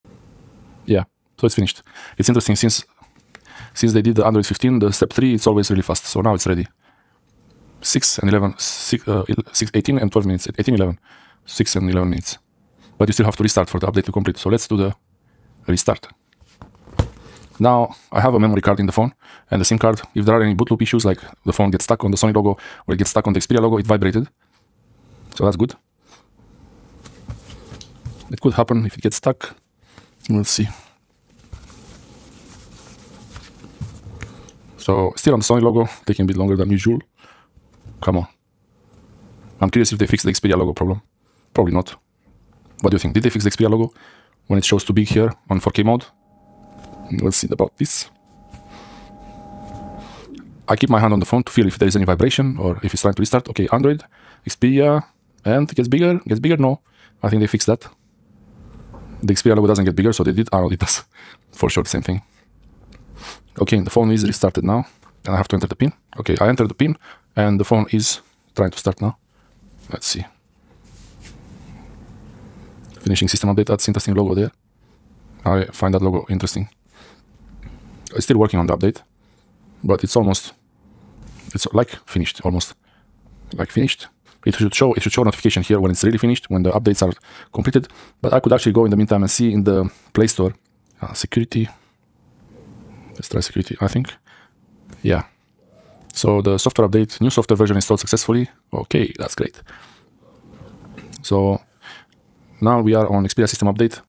Sony Xperia 1 V Android 15 Update 67.2.A.2.60 part 1
This is the first update after the big update bringing Android 15 to Sony Xperia 1V.
There are some issues with the YouTube app freezing and crashing sometimes and hopefully this update will fix some of the problems with the YouTube app.

There is also the problem with the Xperia logo showing too big during a restart when the screen ins in full resolution mode aka. the 4k mode.

What changed, the size of the updates suggests bug fixes especially being the first update received from Sony after a major update to Android 15 already.

Changes the the Sony Xperia apps.
I don't see anything different with the Videography Pro and Photography Pro apps and that's good for me.
Everything still works after updating it.

Videography Pro app not updated:
same version 1.4.2.A.0.5

Photography Pro app not updated:
same version 1.7.2.A.0.8

Music Pro app not updated:
same version 1.1.1.A.0.8

Cinema Pro app not updated:
same version 1.9.2.A.0.3

I will update this description as I find more details or notice any problems and any good changes.

The Videography Pro app, Photo Pro app and Cinema Pro app versions are different after the update. I will use them and update if there's any noticeable difference.
If you notice in the video any difference, let me know please.

What changed in Android 15 for Sony Xperia phones:

Private space for banking apps and any other apps that keeps your private information more secure requiring phone authentication even if unlocked to see the private content area.
Quick Settings menu has a lot of visual changes and new functions.
You can now use the broadcast function from the Settings menu or the Quick settings panel.
Listening to someone else’s shared audio with the headphones paired with your device.
The name of the Headphones Connect app has been changed to “Sony | Sound Connect”.

Sony Xperia 1 V Android 15 update  installing Android 15 update. Installation and steps take a long time to complete.
Xperia restart after update stuck at SONY Logo but went further eventually to the Xperia logo during restart.
You should start a software system update with at least 50% battery.
This shows what to expect when updating the Sony Xperia 1 V system on Android 15 software system update.
Sony Xperia 1 V Software Update 67.2.A.2.60 with 1061.5 MB size
Sony Xperia 1 V XQ-DQ54 Global - EU Model

IMPORTANT READ HERE
If you get a boot loop stuck on the Sony logo on the Xperia logo:
Before you start the update, remove any memory card and have 1 sim only.
better to remove the sim card tray during the update to avoid the phone being stuck on Xperia logo or stuck on Sony logo or worse boot loops.

Xperia system update details:
Update Xperia 1 V Model XQ-DQ54
From 67.2.A.2.41
Update to build number:
67.2.A.2.60
Size: 597.4 MB

At this time, the change log or update history is not yet updated but you can check the link to see what changed in Android 15 after this update
67.2.A.2.60

I updated my phone to this latest version and I can't seem to feel any changes for the good but at least the YouTube app didn't crash yet.
It usually takes some time to crash and show the not responding close YouTube app or wait.

The phone feels the same.
You should definitely check for Google Play Store security updates after this system update to be sure you are on the latest patches for security.

Looks ok to me.

Sony Xperia 1 V Specifications:
Manufacturer: SONY
Model: XQ-DQ54
ROM: 256GB
RAM: 12GB
Color: Black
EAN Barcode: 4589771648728
Sony Xperia 1 V EU model XQ-DQ54
SKU: XQDQ54C0B.EUK